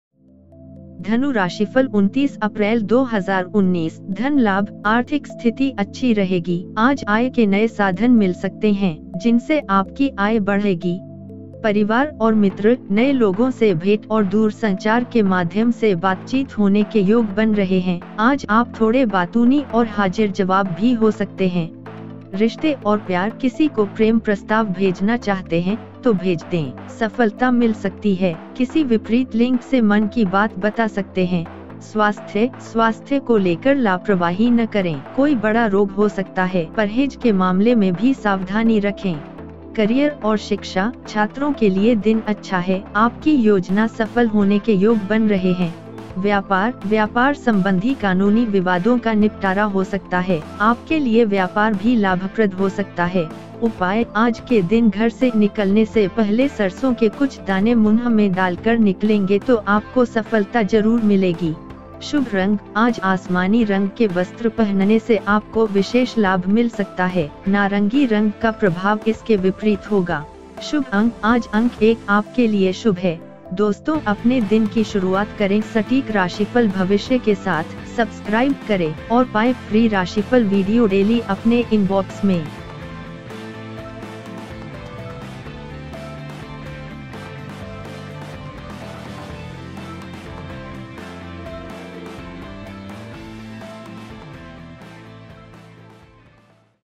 29 April 2019 धनु राशि | आज का राशिफल | Daily Rashifal | Dainik Rashifal | today horoscope
वीडियो में दी गयी जानकारी इंटरनेट पर के आधार पर दी गयी है
Bhagyashali Totke चैनल इसके सच या झूठ होने का दावा नहीं करता.

धन लाभ:- आर्थिक स्थिति अच्छी रहेगी। आज आय के नए साधन मिल सकतें हैं। जिनसे आपकी आय बढ़ेगी।

परिवार और मित्र  :- नए लोगों से भेट और दूरसंचार के माध्यम से बातचीत होने के योग बन रहे हैं। आज आप थोड़े बातूनी और हाजिरजवाब भी हो सकते हैं।

रिश्ते और प्यार  :- किसी को प्रेम प्रस्ताव भेजना चाहते हैं तो भेज दें। सफलता मिल सकती है। किसी विपरीत लिंग से मन की बात बता सकते हैं।

Money Benefit: - Economic conditions will remain good. Today, new sources of income can be found. Which will increase your income.

Family and friends: - Yoga is happening to meet new people and talk through telecommunications. You can be a little talkative and presentable today.

Relationships and Love: - If you want to send a love proposal then send it. Can get success. Can tell the story of the mind with any opposite sex